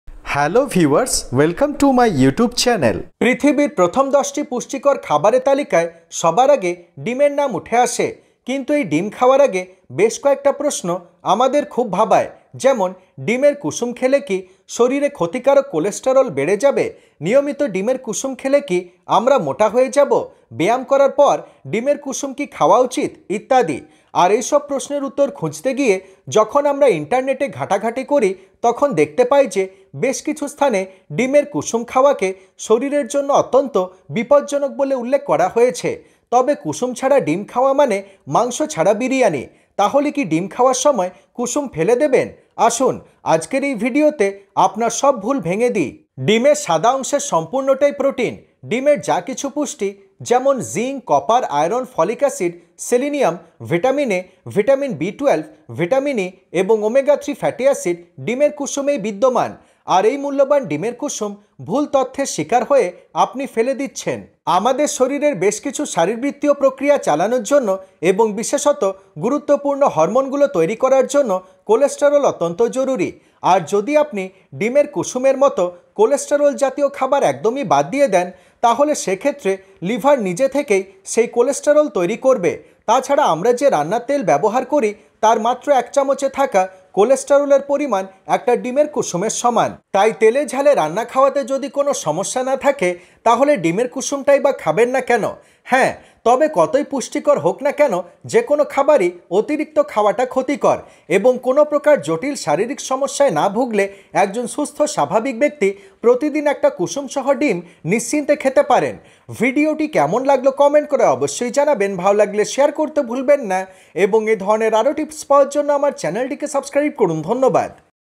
💥99% মানুষ ভুল করছেন💥 ডিমের কুসুম খাওয়া কি ক্ষতিকর | ডিমের কুসুম খেলে কি হয় | ডিম খাওয়ার নিয়ম
পৃথিবীর প্রথম দশটি পুষ্টিকর খাবারের তালিকায় সবার আগে ডিমের নাম উঠে আসে কিন্তু এই ডিম খাওয়ার আগে বেশ কয়েকটা প্রশ্ন আমাদের খুব ভাবায় যেমন:- ডিমের কুসুম খেলে কি শরীরে ক্ষতিকারক কোলেস্টেরল বেড়ে যাবে? নিয়মিত ডিমের কুসুম খেলে কি আমরা মোটা হয়ে যাব?
ব্যায়াম করার পর ডিমের কুসুম কি খাওয়া উচিত? ইত্যাদি। আর এইসব প্রশ্নের উত্তর খুঁজতে গিয়ে যখন আমরা ইন্টারনেটে ঘাঁটাঘাঁটি করি তখন দেখতে পাই যে বেশ কিছু স্থানে ডিমের কুসুম খাওয়াকে শরীরের জন্য অত্যন্ত বিপদজনক বলে উল্লেখ করা হয়েছে। তবে কুসুম ছাড়া ডিম খাওয়া মানে মাংস ছাড়া বিরিয়ানি। তাহলে কি ডিম খাওয়ার সময় কুসুম ফেলে দেবেন? আসুন আজকের এই ভিডিওতে আপনার সব ভুল ভেঙ্গে দিই।